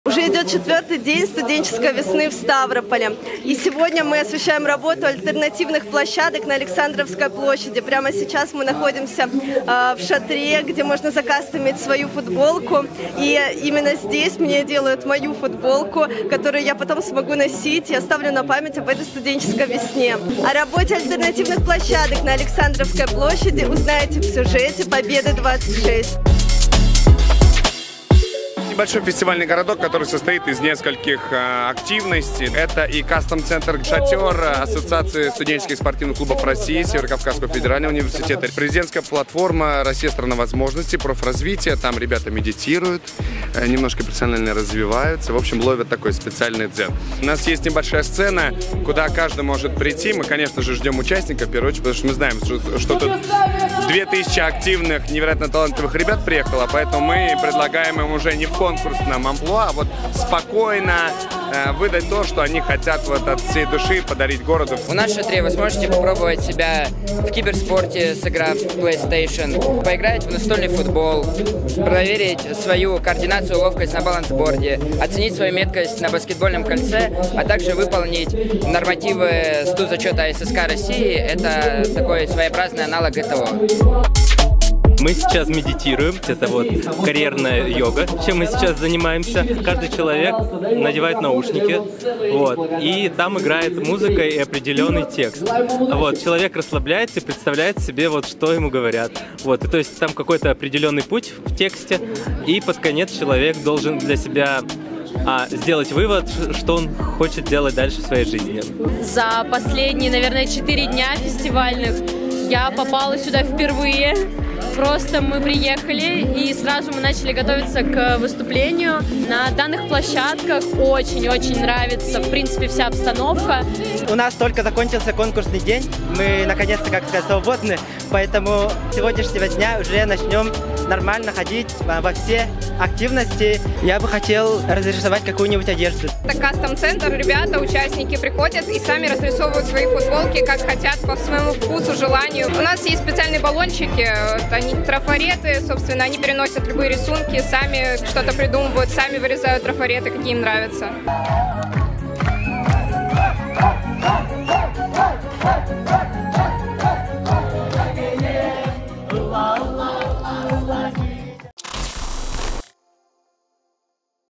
Площадки «Российской студвесны» новости Ставропольского края СКФО Юга России лучшее видео Победа26
С первого дня фестиваля XXXII «Российская студенческая весна» работают альтернативные площадки на Александровской площади в Ставрополе. На них представлен ряд активностей, где могут провести время участники фестиваля и местные жители. Возможностью воспользовалась и корреспондент «Победы26», подробности — в видеосюжете.